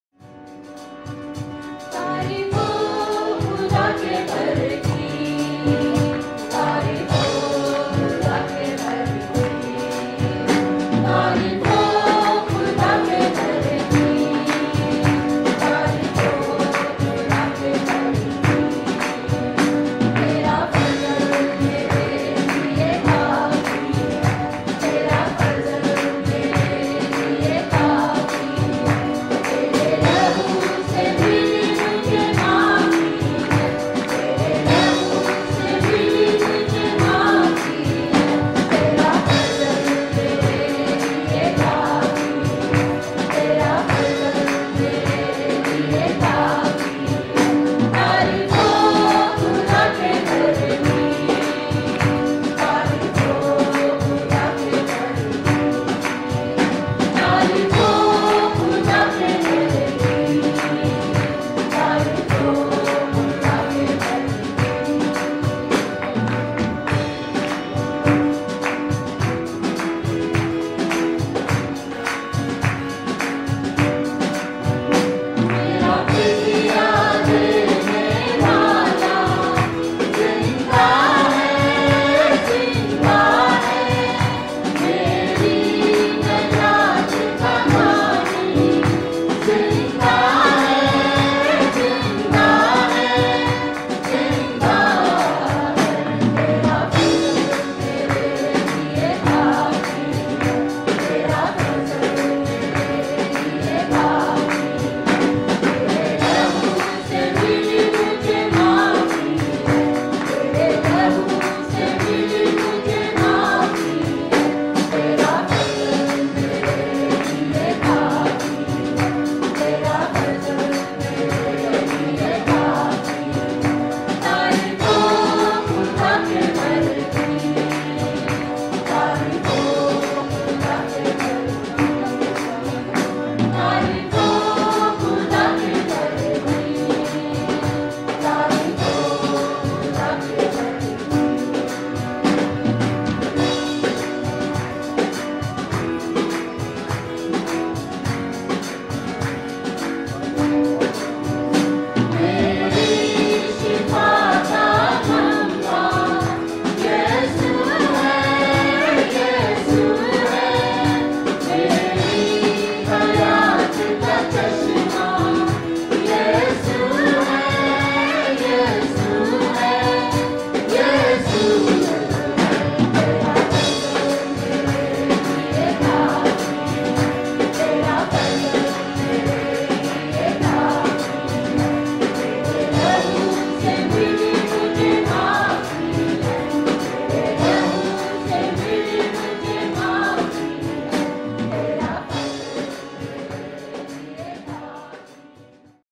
Asian Choir | SCOG Anniversary
Group song by our Asian choir at SCOG's 32nd Anniversary Celebration.